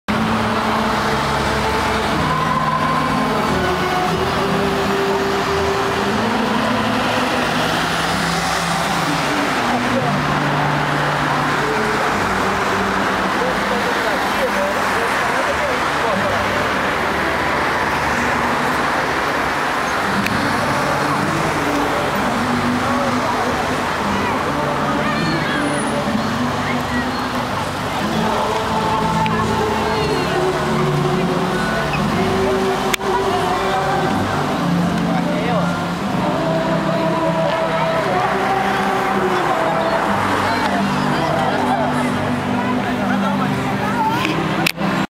イビラプエラ公園の巨大クリスマスツリー
年末年始にかけて、サンパウロのイビラプエラ公園に巨大クリスマスツリーが出現します。
電飾で飾られており、夜は素晴らしいイルミネーションで、クリスマス気分を盛り上げてくれます。